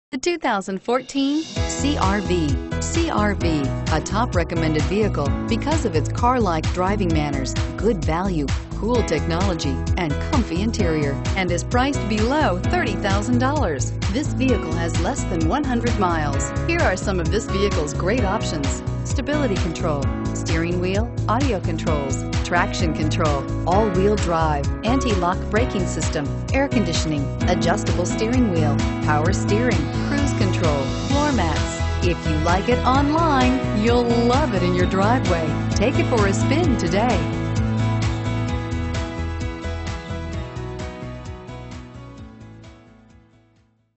2014 HONDA CR-V Indianapolis, IN 2CR3884
2014 HONDA CR-V Indianapolis, IN
Stock# 2CR3884

The 2014 Honda CR-V has a modern aerodynamic look, with sweeping lines, exciting body sculpting, and a dramatic profile. It has enough attitude and ability to get you around town and then tackle the open road and the great outdoors. The CR-V comes equipped with a 2.4-Liter i-VTEC 4-Cylinder engine with 185 Horsepower and a 5-Speed Automatic Transmission. It has a rigid unit-body structure, coupled with taut suspension tuning, which give it impressive handling characteristics. For ultimate 4-season security, the 2014 CR-V is available with Honda's Real Time All-Wheel Drive with Intelligent Control System that smoothes out initial acceleration and can transfer up to 100 percent of torque to the rear wheels if necessary. The CR-V is very fuel-efficient and can get up to an EPA estimated 31 MPG Highway when in two-wheel drive. Hit the green ECON button, and the vehicle makes adjustments to its systems so they can be more efficient. Then you get feedback as you drive from Eco Assist to let you know how efficient your driving is. Honda has placed bars around the speedometer, and the longer you keep them in the green, the farther you can go on a tank of gas. The 5-passenger CR-V remains modestly sized on the outside, but is extremely roomy inside, with a rear seat that accommodates adults and a proper-sized cargo area. Special attention is paid to driver comfort, with supportive front seats that flank an accommodating center console punctuated by two big cup holders. A large covered center stow bin houses USB and auxiliary inputs as well as a 12-volt power point. Finally, its large single-piece liftgate, coupled with a significantly lower lift-over height, eases loading and unloading chores. Check out the 2014 Honda CR-V Today!

All Wheel Drive, Power Steering, ABS, 4-Wheel Disc Brakes, Brake Assist, Steel Wheels, Tires - Front All-Season.